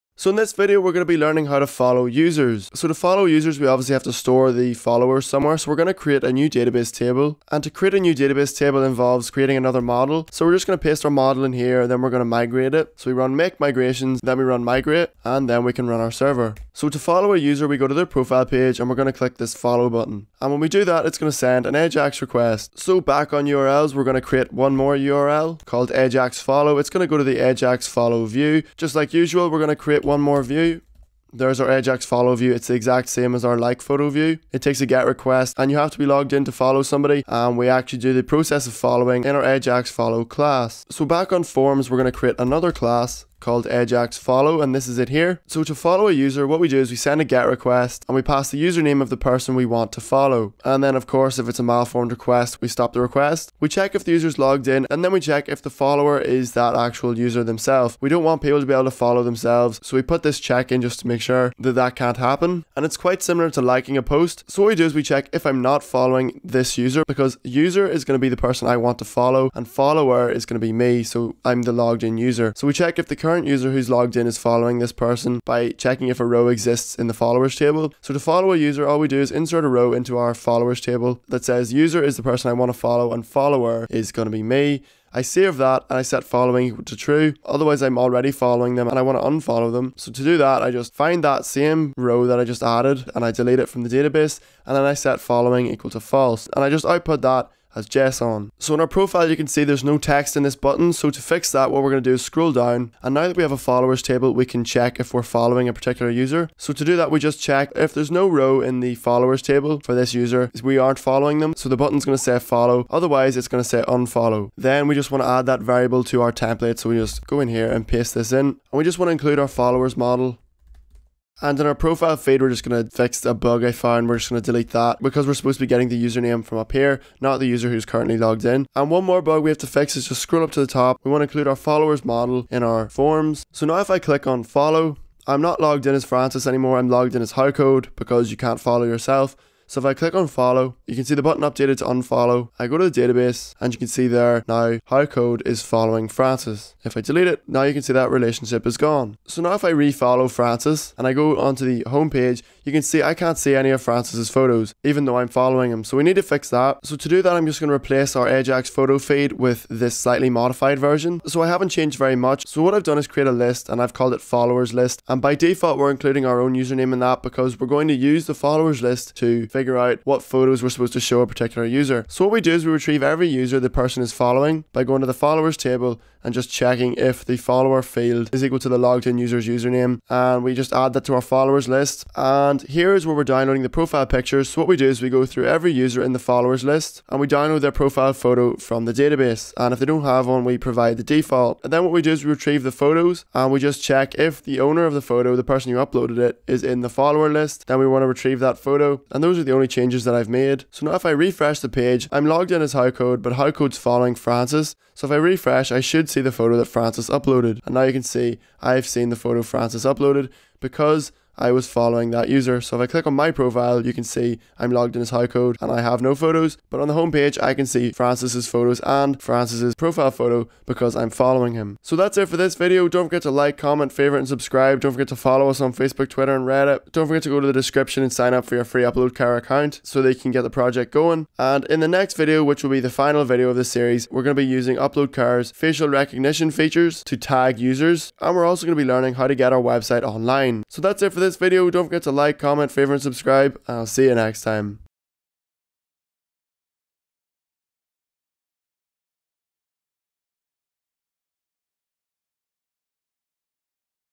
Make your Own Instagram - Following Users (9/10)
In this video we'll be learning how to follow users and how to display the photos from users we're following in our photo feed!